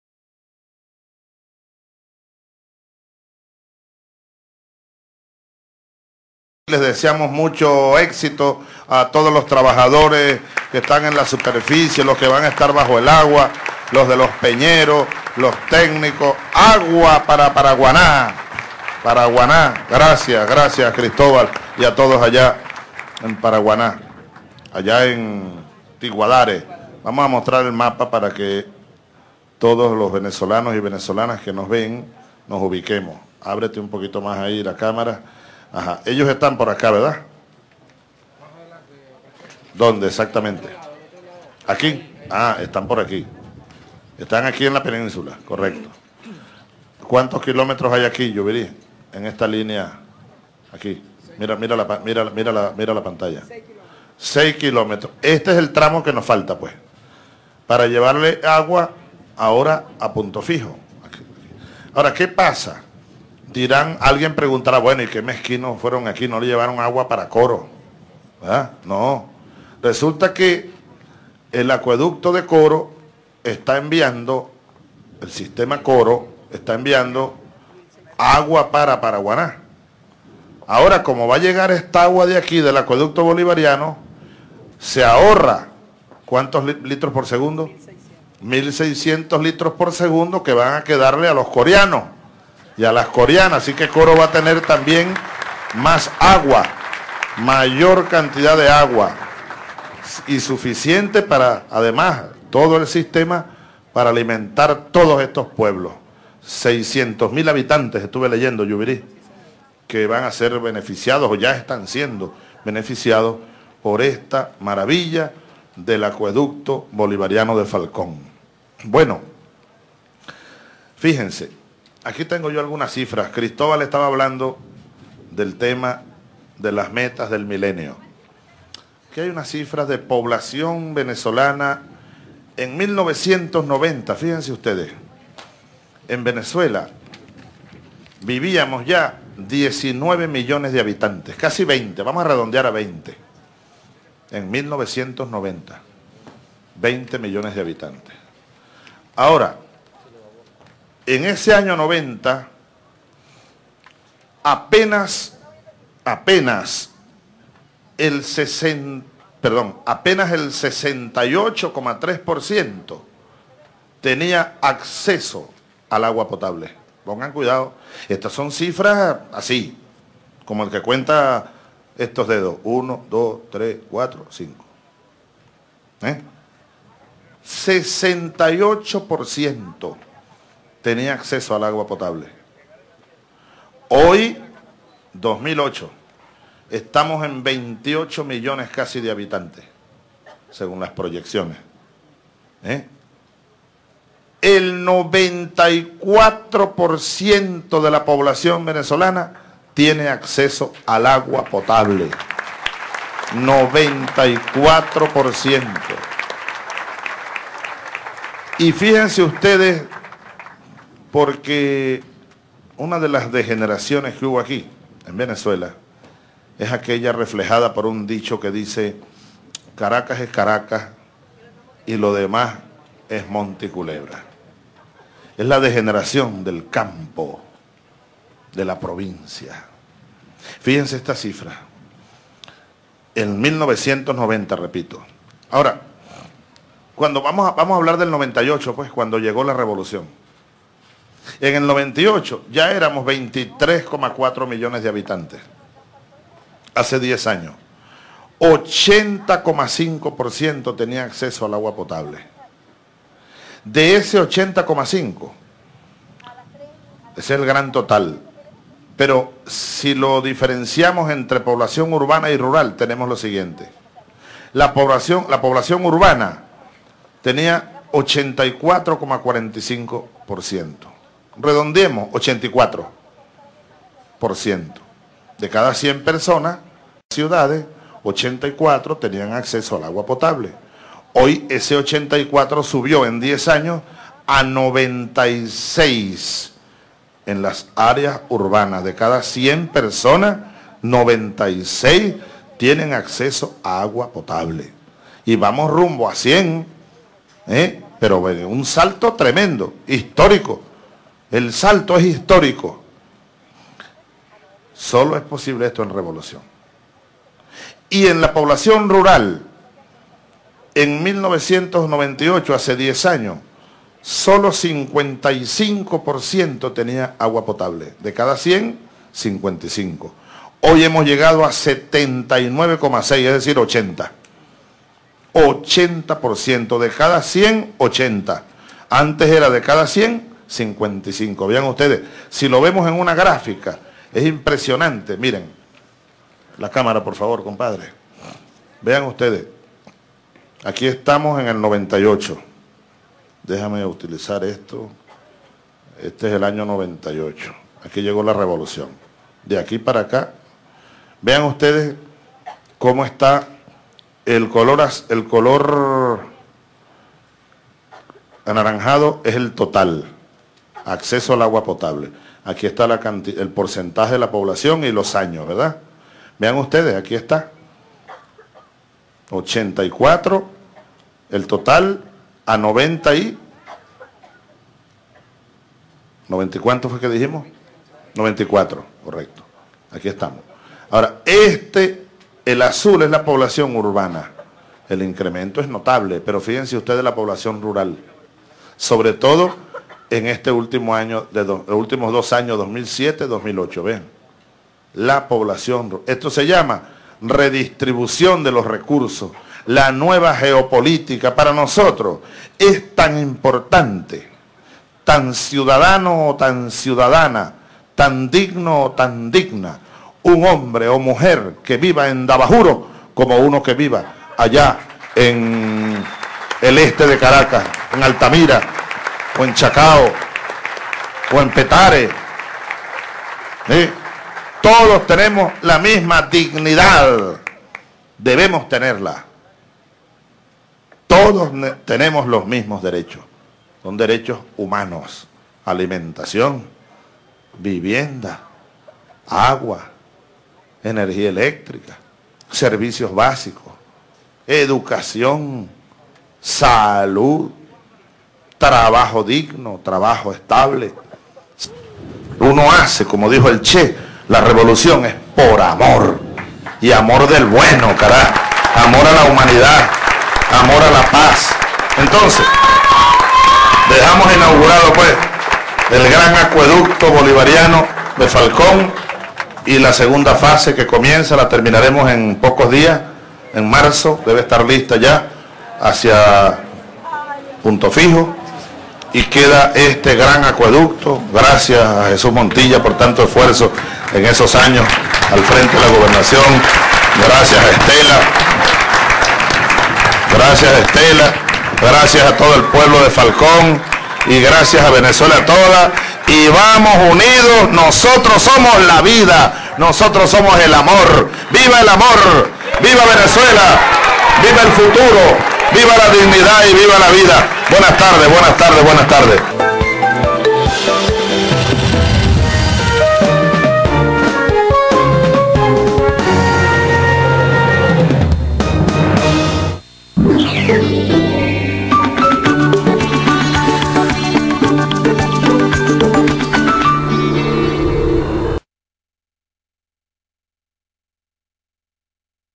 8 Feb 2009 94% de la población venezolana tiene acceso al agua
Venezuela ha dado un salto histórico: 94% de la población venezolana tiene acceso al agua.